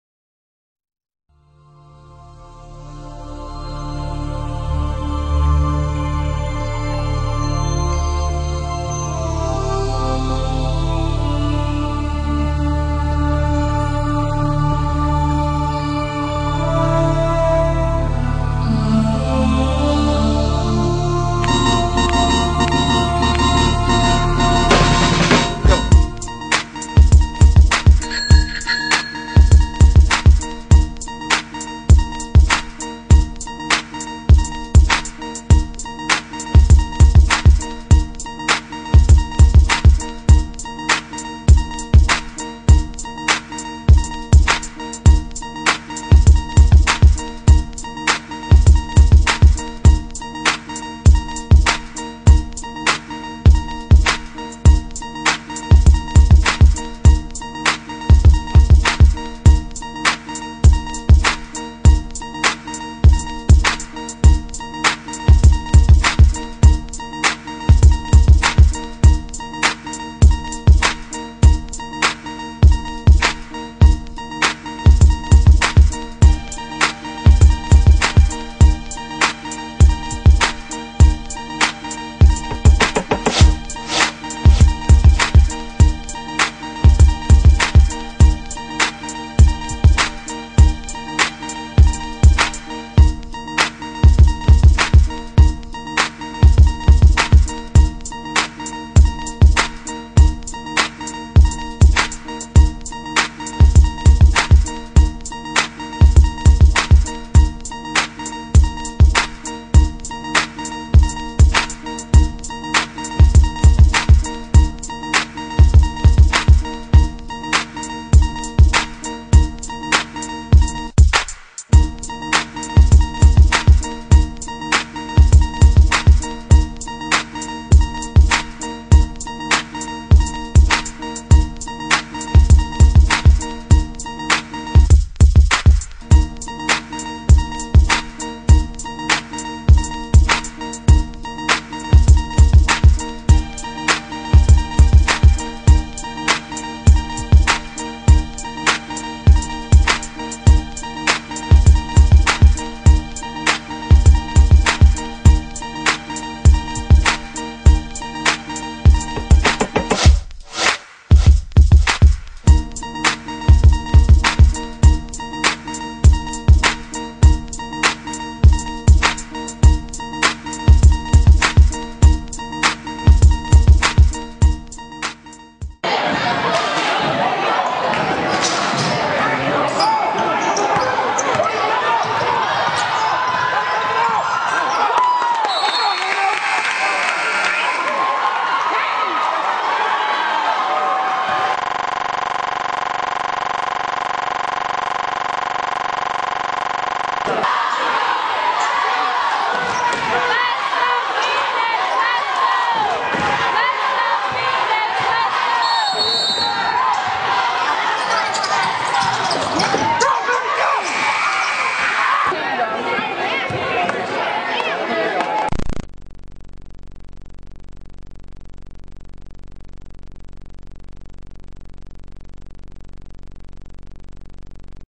RENAISSANCE VS. HENRY FORD ***PHOENIXHOOPS.NET***
PSL WEST BATTLE ON 1/9/09

FORD COMES OUT ON TOP 59-57

-DELANO COLLINS LED THE PHOENIX WITH 19 PTS
**RENAISSANCE HIGHLIGHTS ONLY**